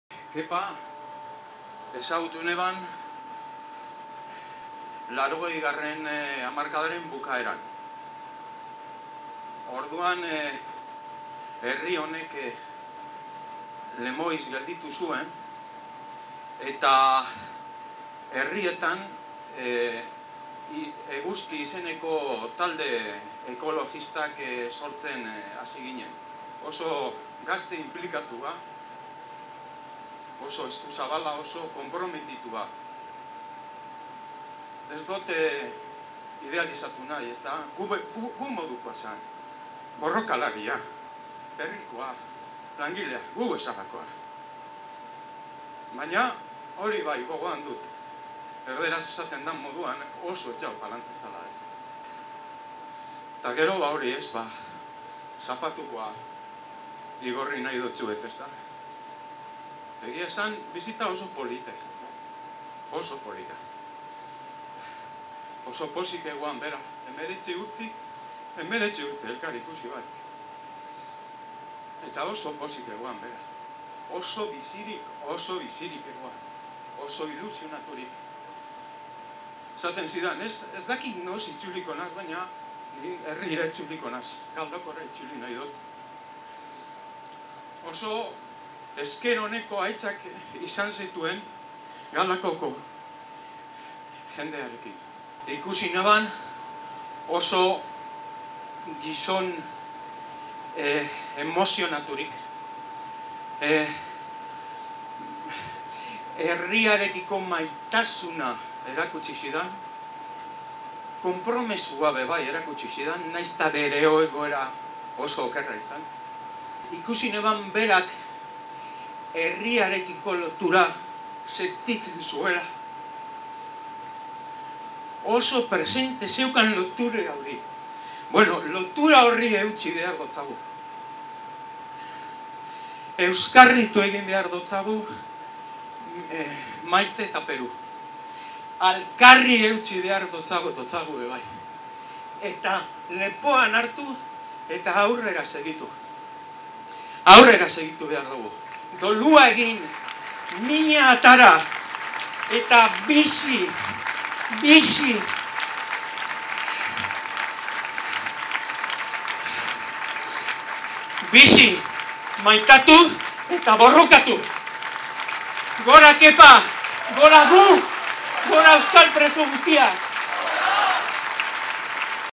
AgurEtaOhoreKepa Kepa Del Hoyo-ren lagunaren hitzak
Kepa Del Hoyo euskal presoa bisitatu zuen azken lagunaren hitzak Galdakaoko hilerrian.